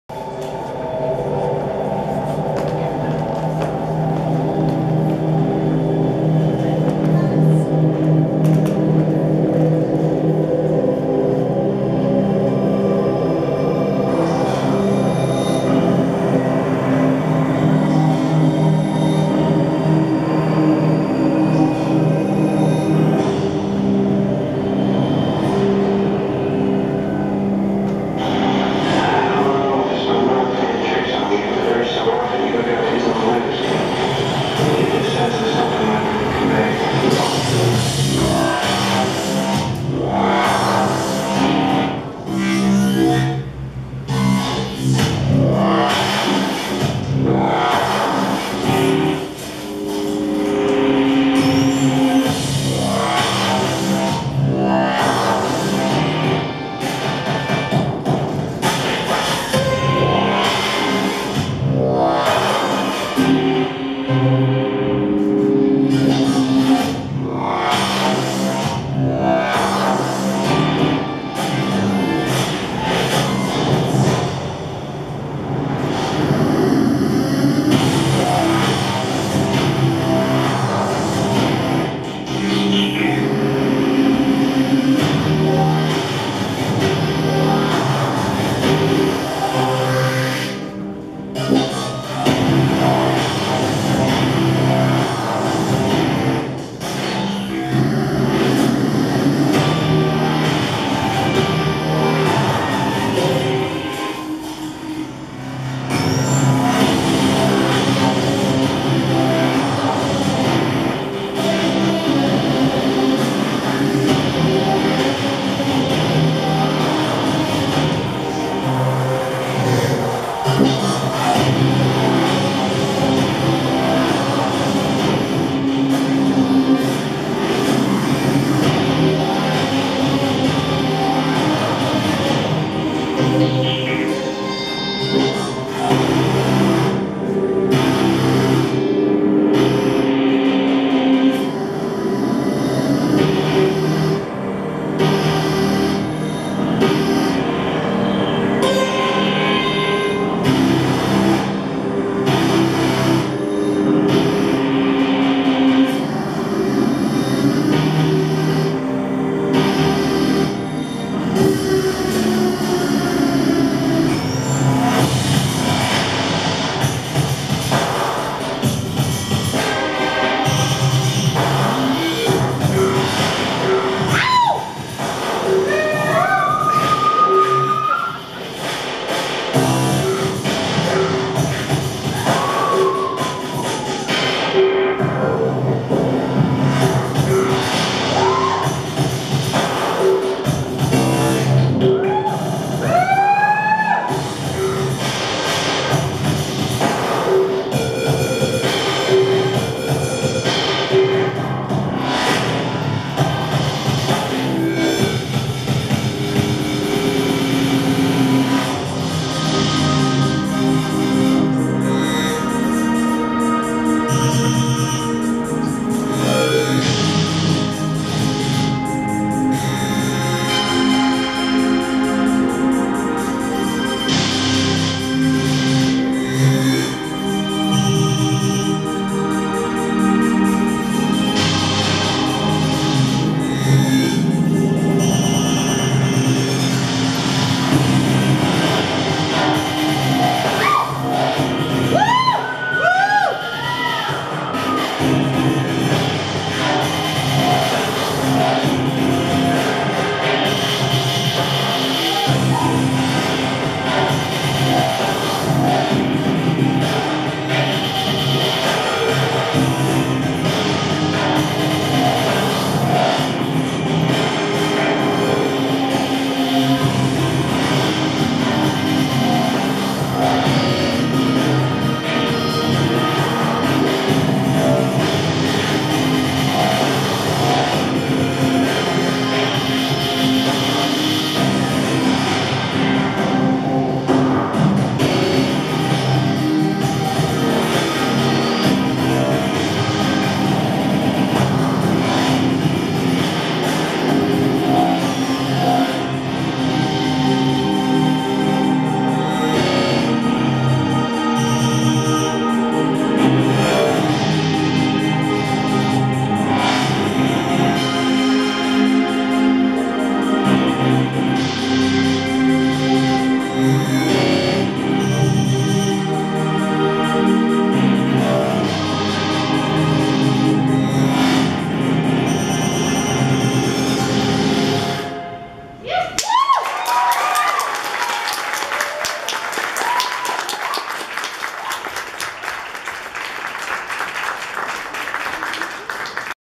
Wicked Moon 2014
Here's the improvisation that I did at Wicked Moon Halloween Belly Dance show